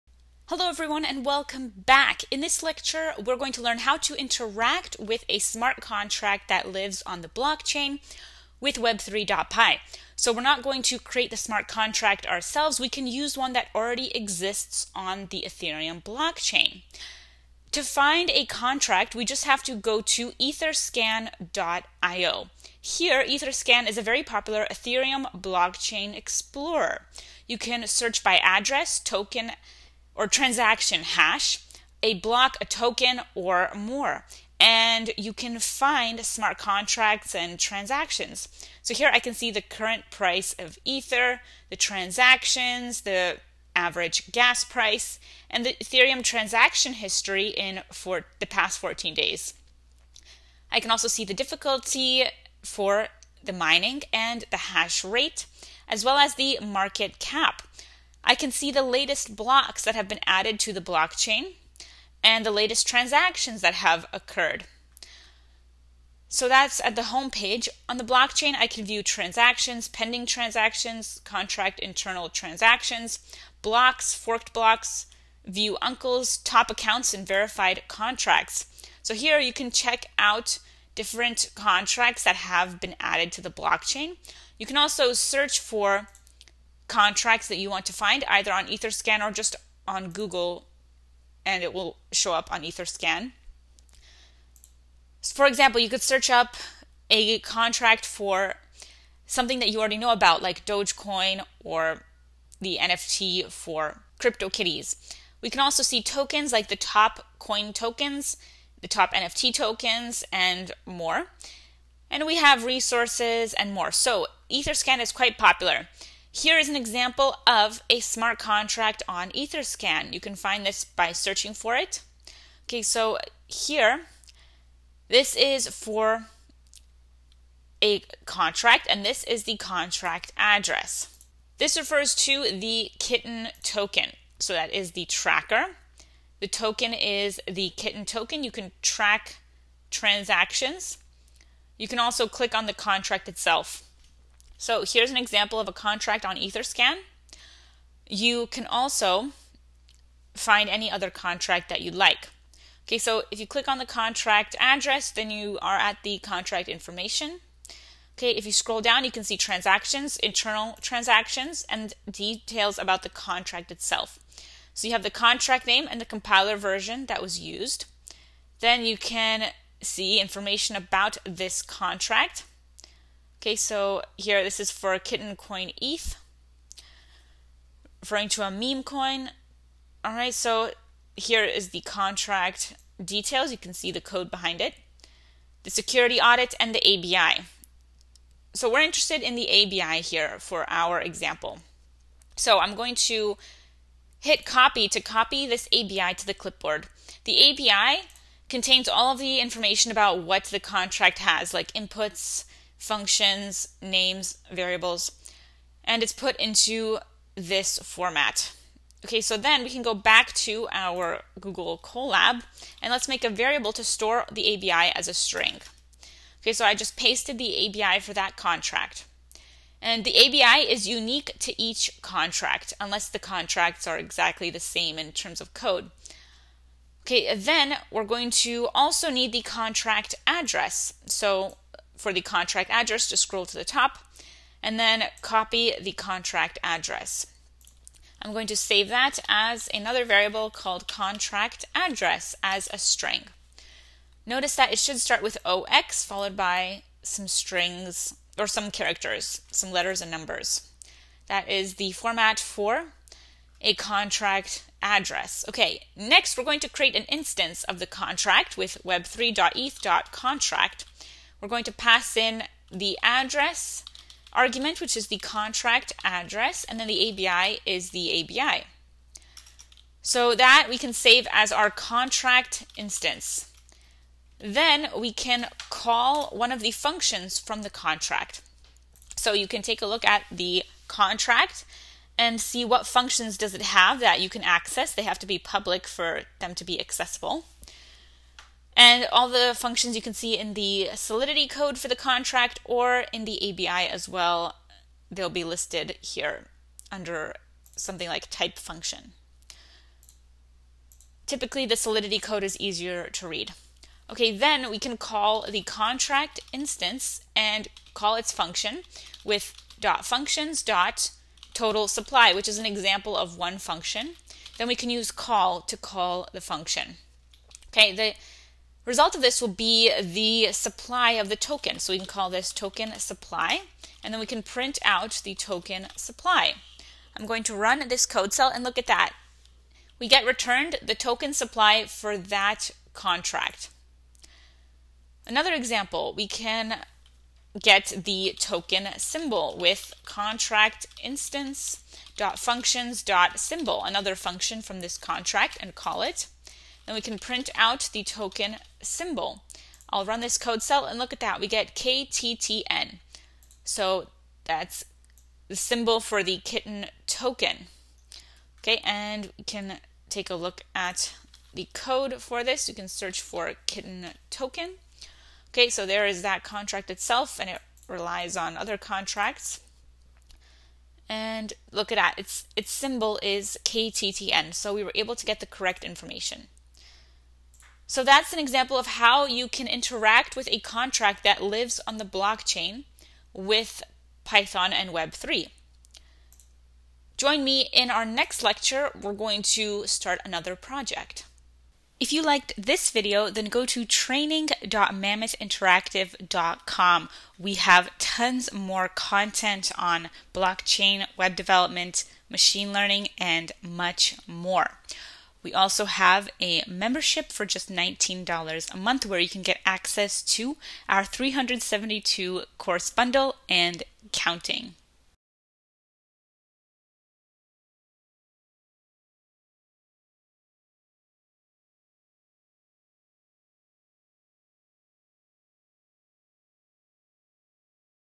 Interact with Blockchain Contract with Web3 Python
Interact with blockchain contract with Web3 Python on Google Colab Jupyter Notebook!

00:00 Ethereum blockchain explorer Etherscan
01:55 Smart contract on Etherscan
03:05 Get smart contract ABI from Etherscan
03:46 Get smart contract address from Etherscan
05:05 Interact with smart contract from Etherscan with web3.py
06:31 Where to go from here

Tools:
● Python 3.7.12
● Web3.py 5.28.0

● Web3 explained ● Web3.py ● Ethereum beginner tutorials ● Blockchain programming beginners tutorial